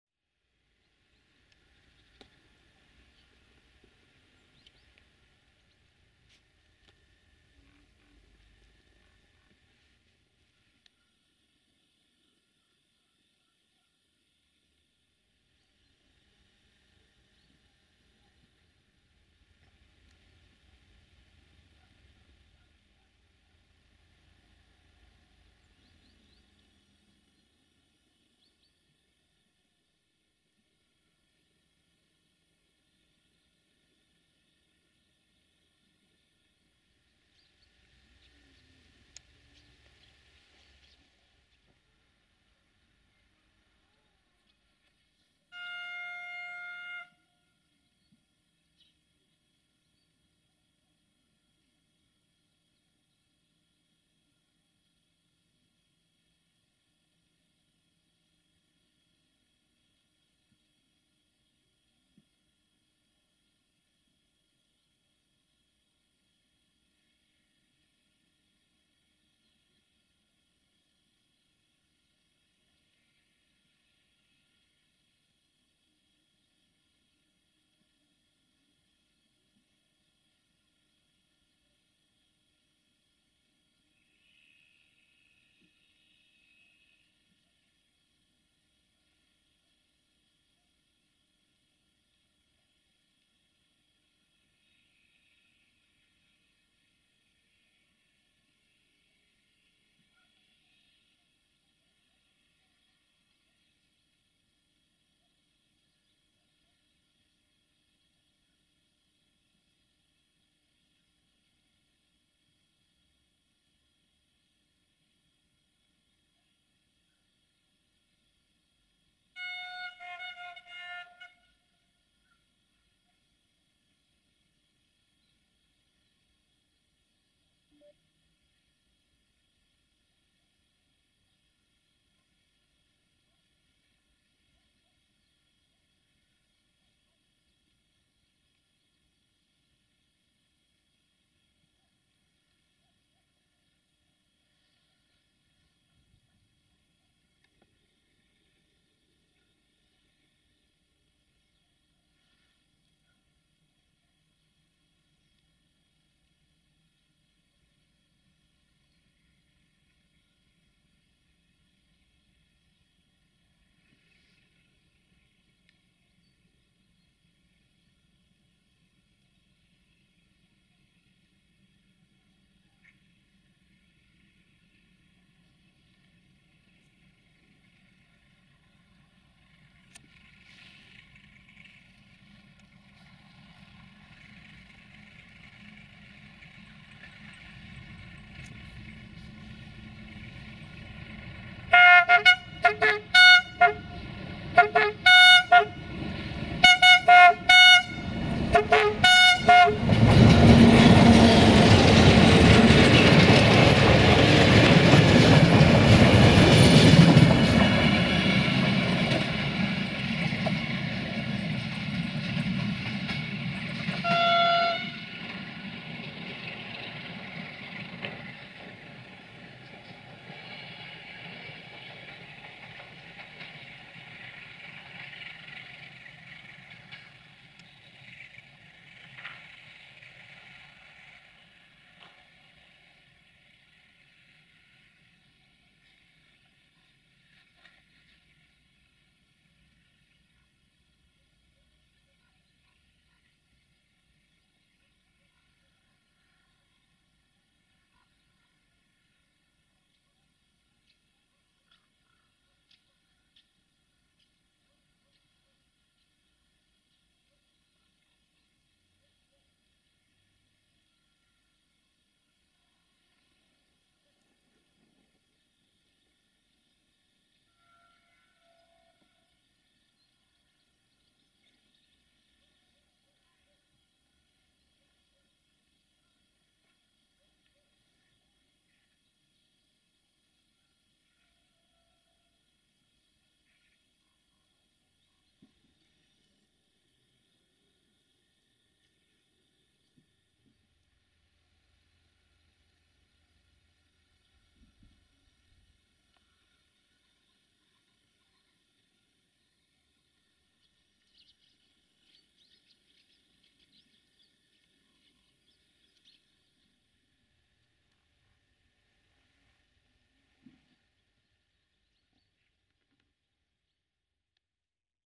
Horns Show de Goarne By 060-DA 60-0809-3 & Mecanici Super Engineers in Halmeu - 03 November 2020
00:46 R4406 Halmeu-Porumbești-Micula-Satu Mare - 060-DA/LDE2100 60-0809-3 RO-SNTFC

03 November 2020